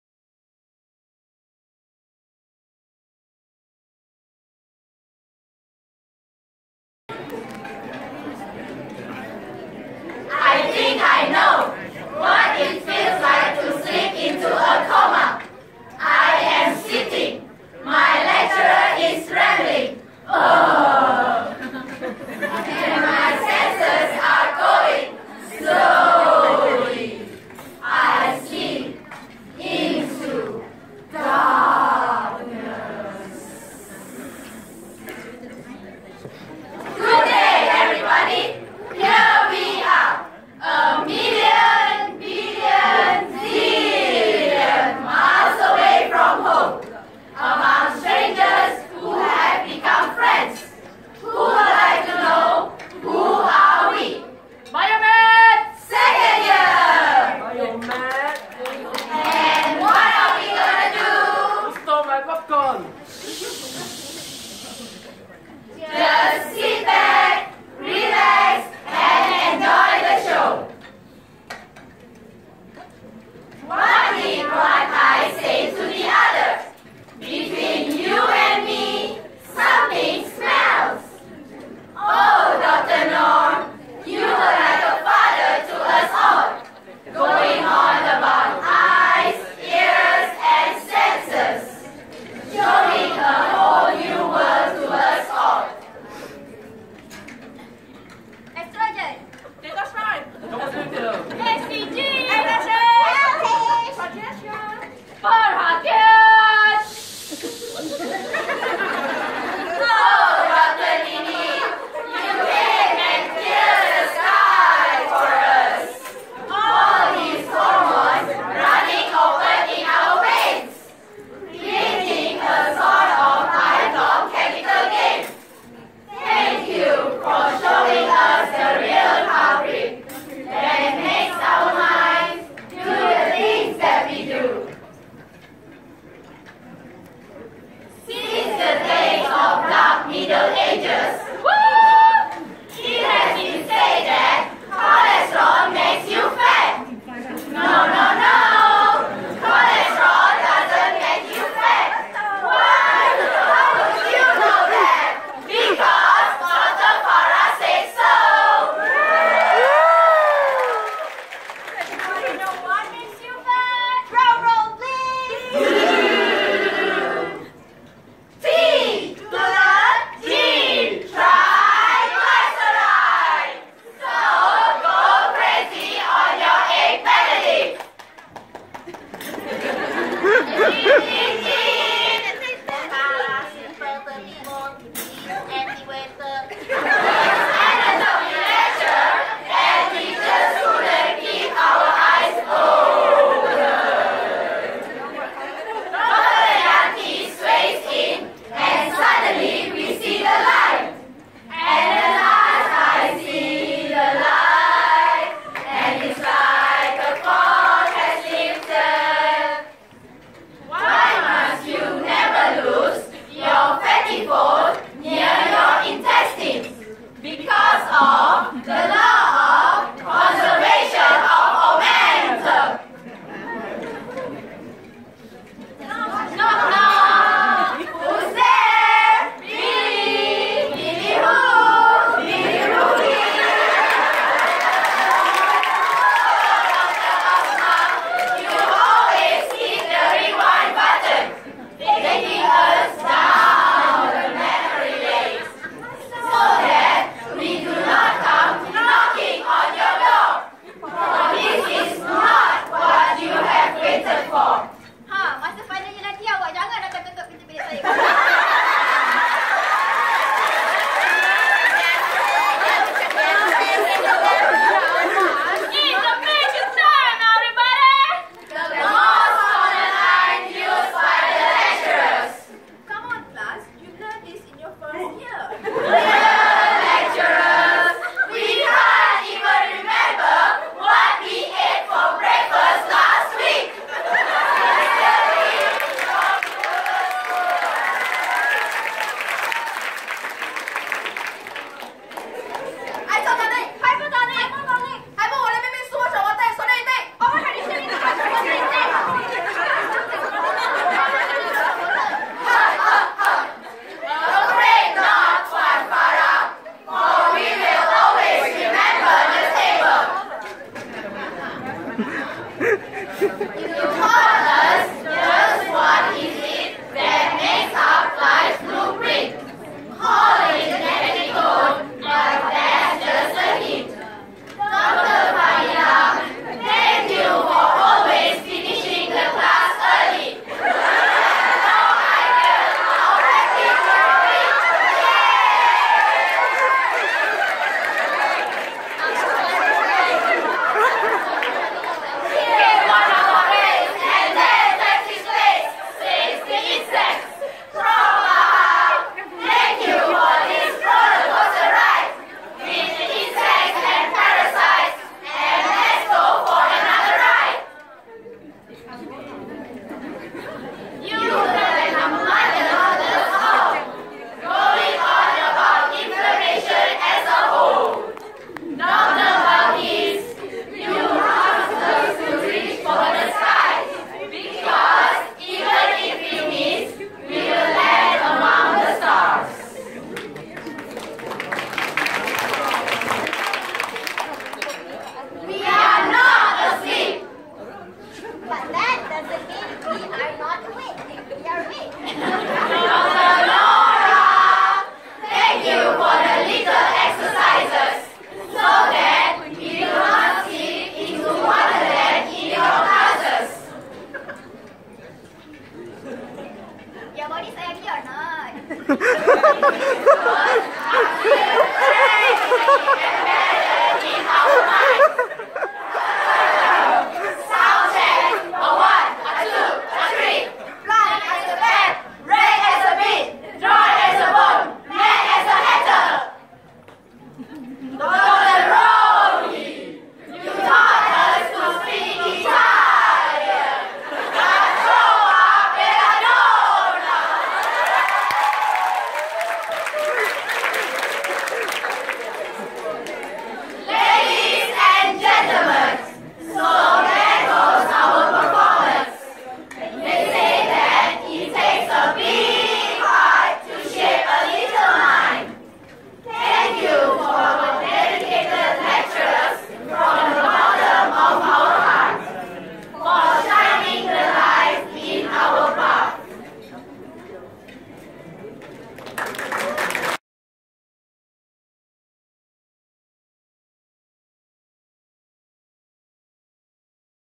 Choral Speaking
A mind-blowing performance by Biomed second year 2016 during Biomed Annual Dinner.
Just sit back, relax and enjoy the show :)